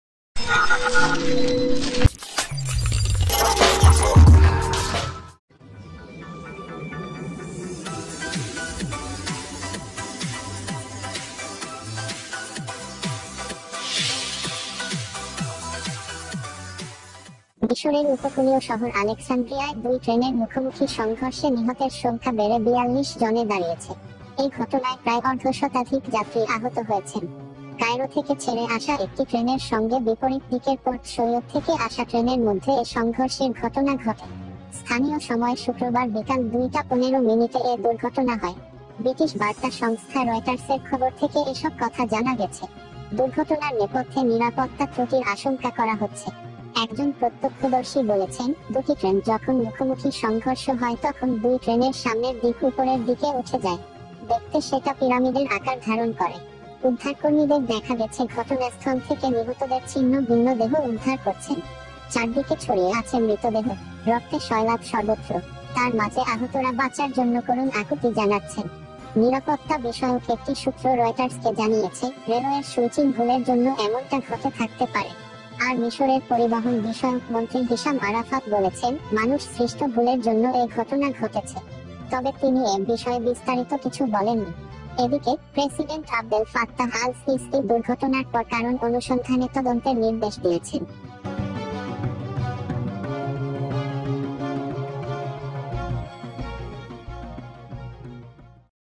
42 killed in collision between two trains in Egypt - Dhakatimes24 by-- Fornews
মিশরে দুই ট্রেনের মুখোমুখি সংঘর্ষে নিহত ৪2   Dhakatimes24